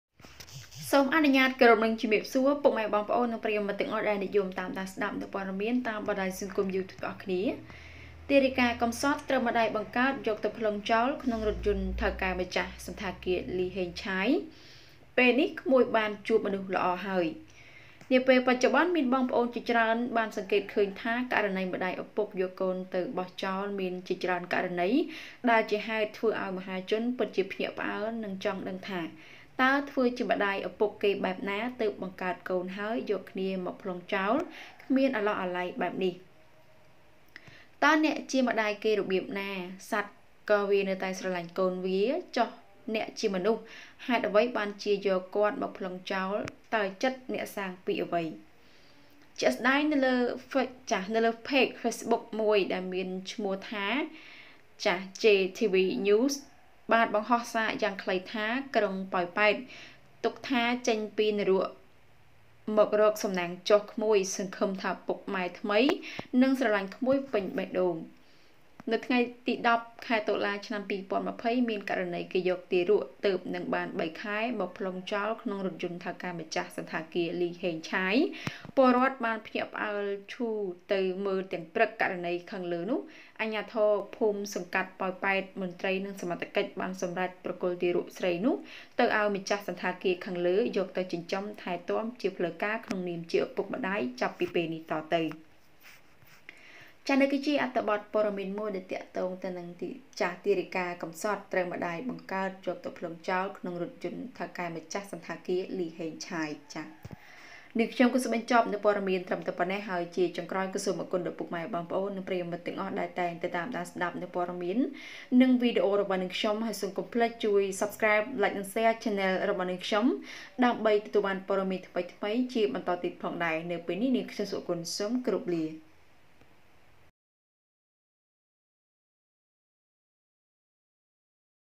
ទារិកាកំសត់ ត្រូវម្តាយបង្កើតយកទៅផ្លុងចោល ក្នុងរថយន្ត ថៅកែម្ចាស់សណ្ឋាគារលី​ ហេងឆាយ ពេលនេះ ក្មួយបានជួប
ទារិកាកំសត់ ត្រូវម្តាយបង្កើតយកទៅផ្លុងចោល ក្នុងរថយន្ត ថៅកែម្ចាស់សណ្ឋាគារលី​ ហេងឆាយ ពេលនេះ ក្មួយបានជួបមនុស្សល្អហើយ សង្ឃឹមថា...

Thanks!